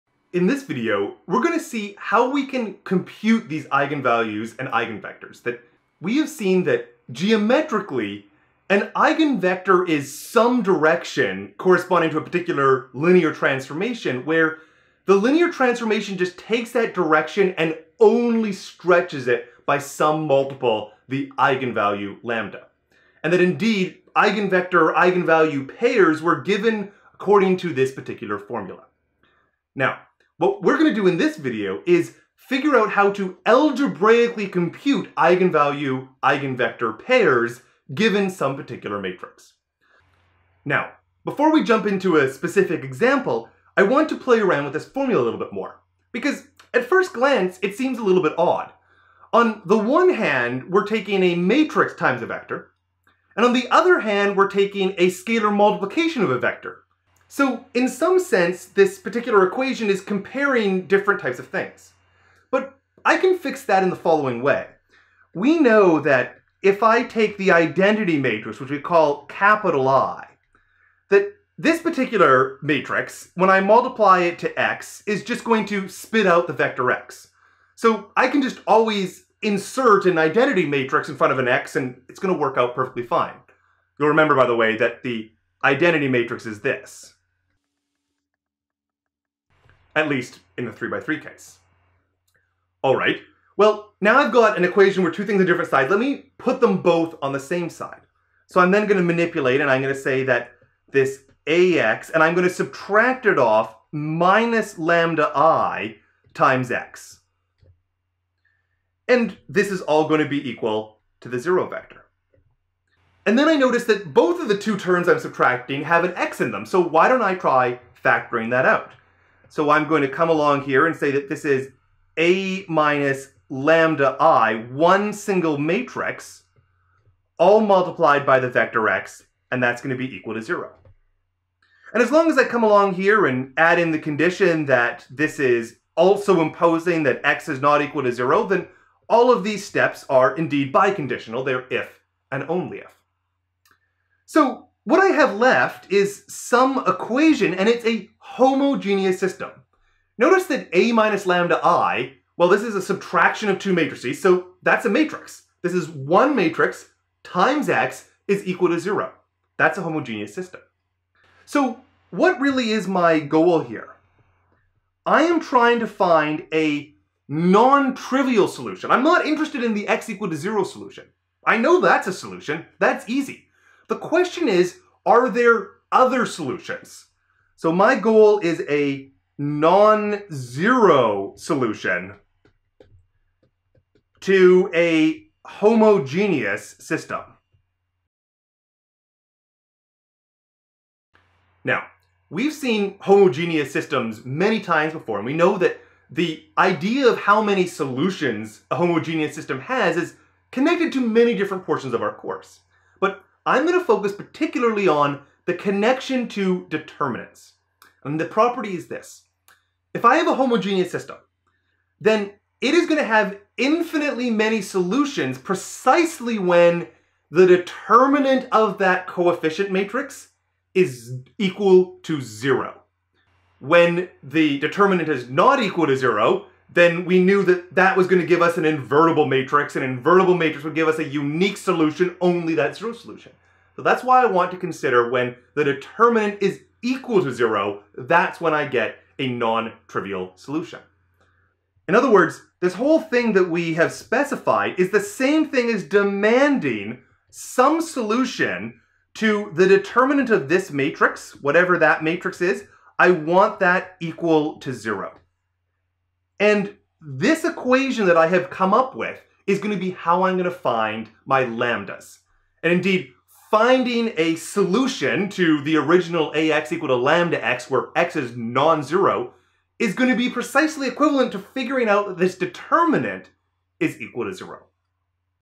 Using determinants to compute eigenvalues & eigenvectors
Motivated by the geometric picture of the previous video, we rewrite the main eigenvalue-eigenvector formula in terms of determinants.

This video is part of a Linear Algebra course taught at the University of Cincinnati.